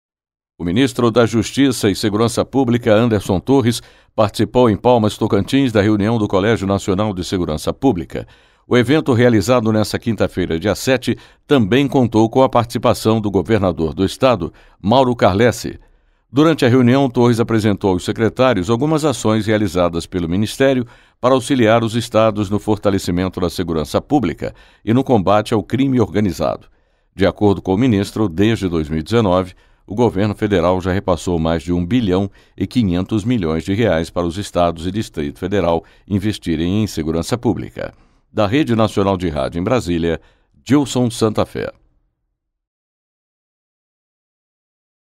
08/10/2021 - MINISTRO DA JUSTIÇA APRESENTA AÇÕES PARA SEGURANÇA PÚBLICA
Em Palmas (TO), MJSP apresentou medidas para auxiliar os estados no fortalecimento da segurança pública e no combate ao crime organizado.

O ministro da Justiça e Segurança Pública, Anderson Torres, participou, em Palmas (TO), da reunião do Colégio Nacional de Segurança Pública. O evento, realizado nessa quinta-feira (7), também contou com a participação do governador do estado, Mauro Carlesse.

Durante a reunião, Torres apresentou aos secretários algumas ações realizadas pelo Ministério para auxiliar os estados no fortalecimento da segurança pública e no combate ao crime organizado.

De acordo com o ministro , desde 2019, o governo federal já repassou mais de 1 bilhão e 500 milhões de reais para os estados e Distrito Federal investirem em segurança pública.

Da Rede Nacional de Rádio, em Brasília, Dilson Santa Fé

registrado em